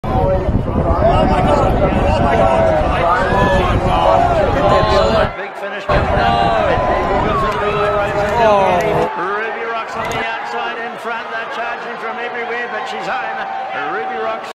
Guy ALMOST DIES FROM Stupid STUNT 🐎🏆😱
Guy runs onto horse track and narrowly escapes getting hit by horse!! Crazy!!